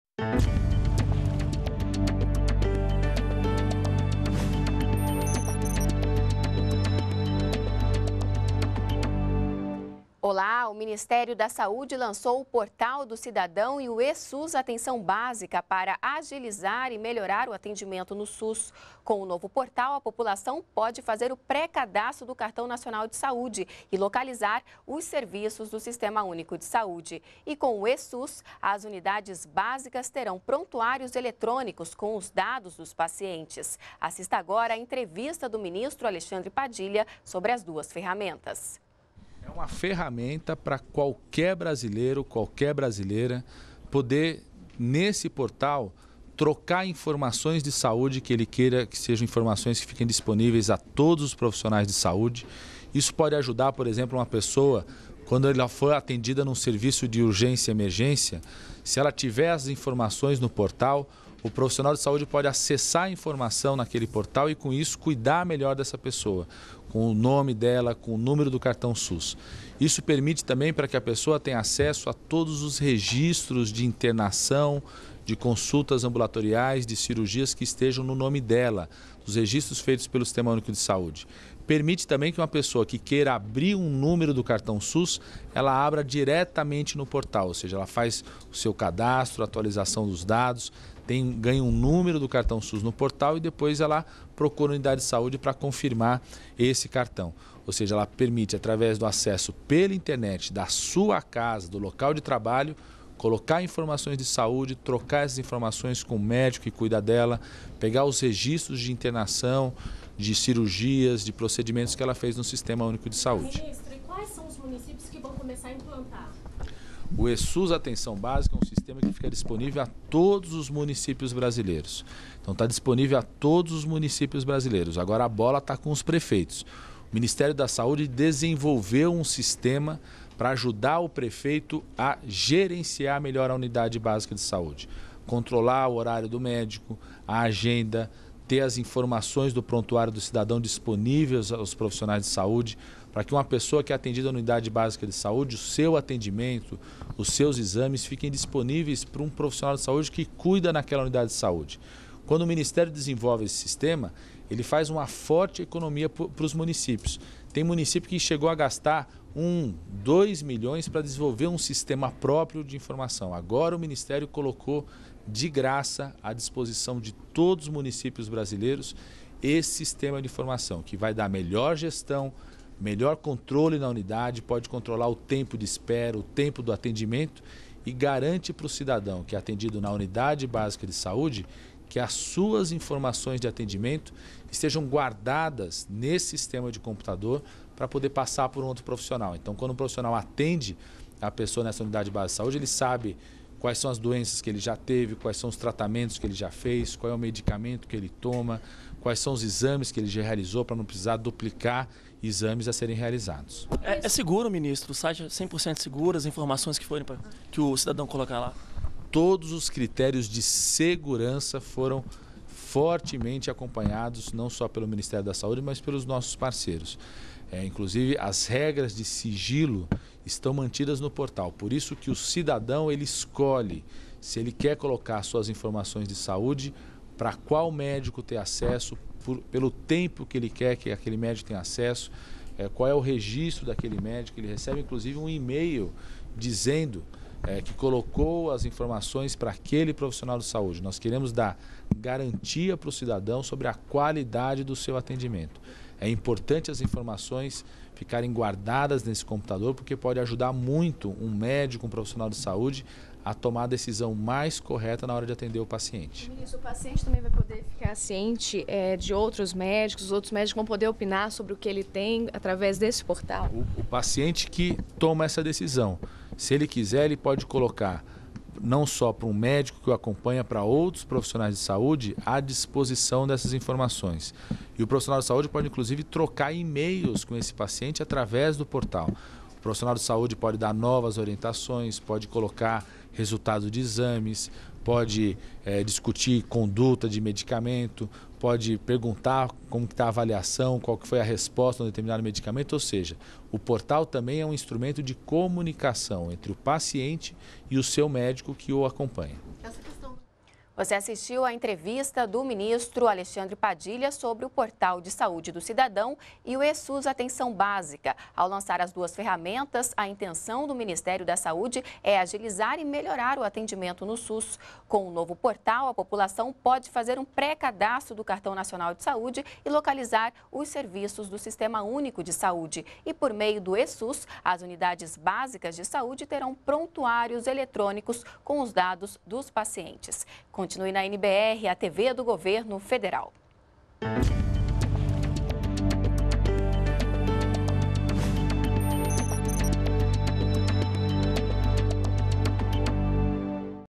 Ministério da Saúde lança Portal do Cidadão e E-SUS para melhorar atendimento
ATIVIDADES DO GOVERNO -- 05.02.13: O Ministério da Saúde lançou, nesta terça-feira (5), o Portal de Saúde do Cidadão e o E-SUS Atenção Básica. As ferramentas foram desenvolvidas com o objetivo de agilizar e qualificar o atendimento e o tratamento dos usuários do Sistema Único de Saúde (SUS). O ministro da Saúde, Alexandre Padilha, explica como as ferramentas serão úteis aos profissionais da área, unidades básica de saúde e população.